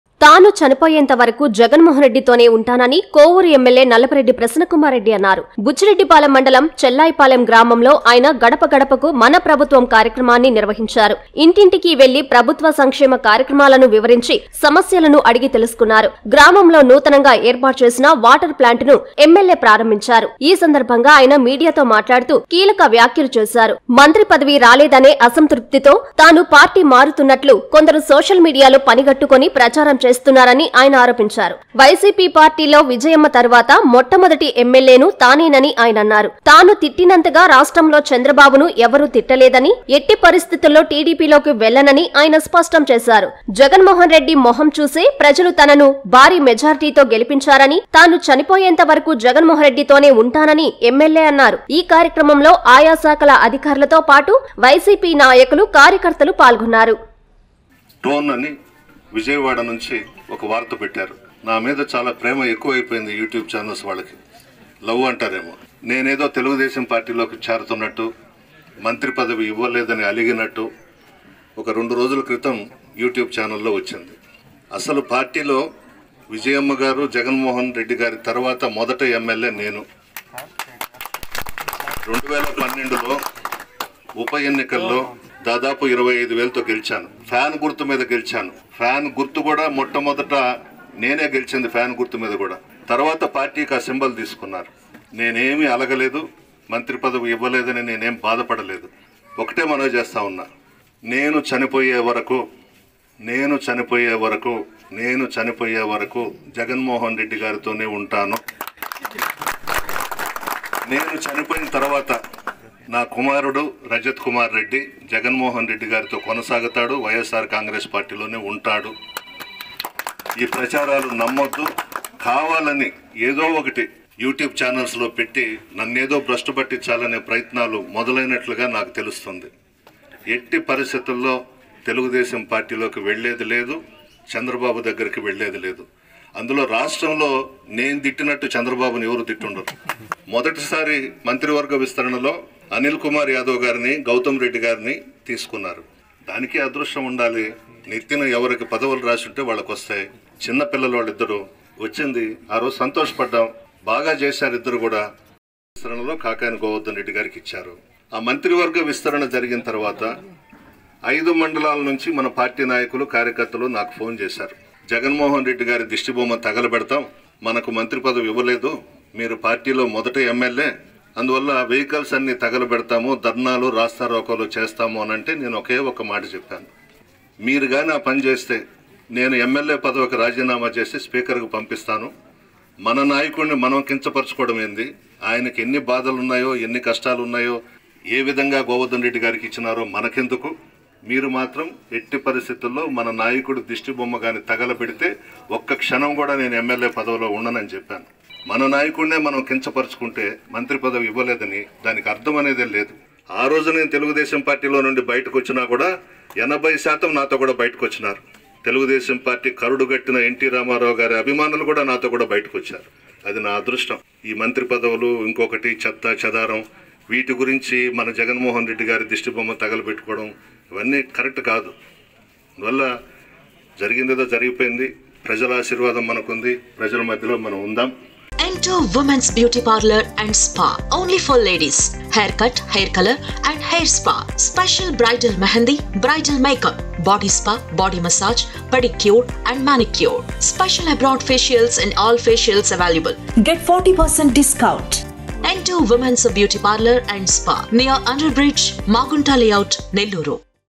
నా తుది శ్వాస విడిచే వరకు జగన్మోహన్ రెడ్డి తోనే ఉంటా - ఎమ్మెల్యే ప్రసన్నకుమార్ రెడ్డి
చంద్రబాబును నాలా ఎవరు తిట్టలేదు..!
చనిపోయేంతవరకు జగన్మోహన్ రెడ్డి తోనే ఉంటా -  ఎమ్మెల్యే ప్రసన్నకుమార్ రెడ్డి